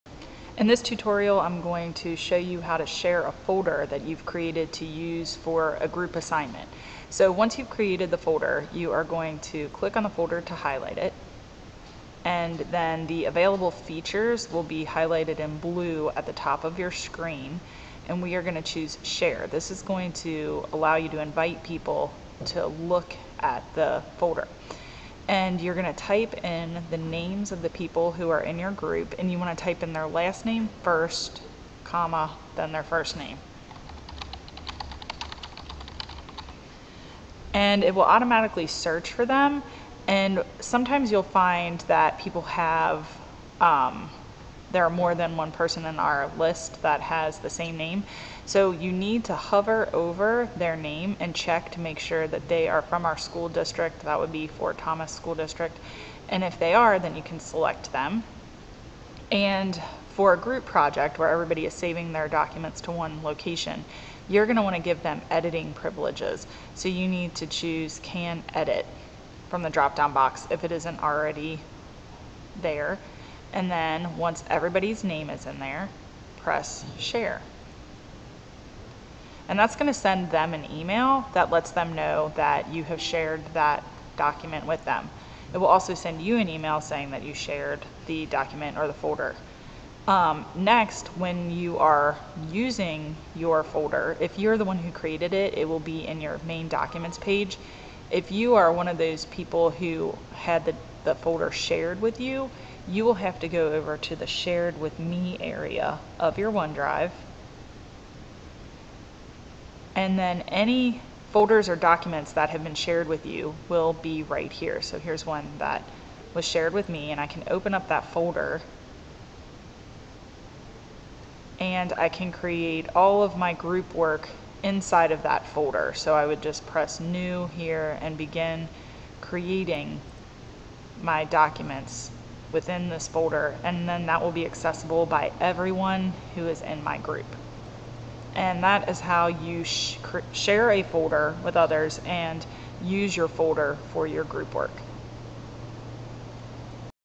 How to Share a OneDrive Folder for Group Work
Learn how to organize your group's work in a folder, accessible by all students.